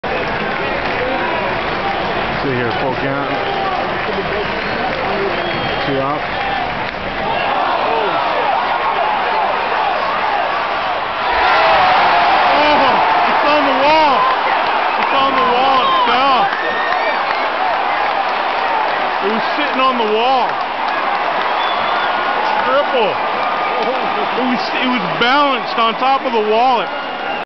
Johnny Damon leaves wall ball sitting after left field body slam
Kevin Youkilis goes deep to left field where Johnny Damon slams into the wall, and leaves the ball sitting on top of the left-field wall...
07.04.08. Top of the 3rd, Yankee Stadium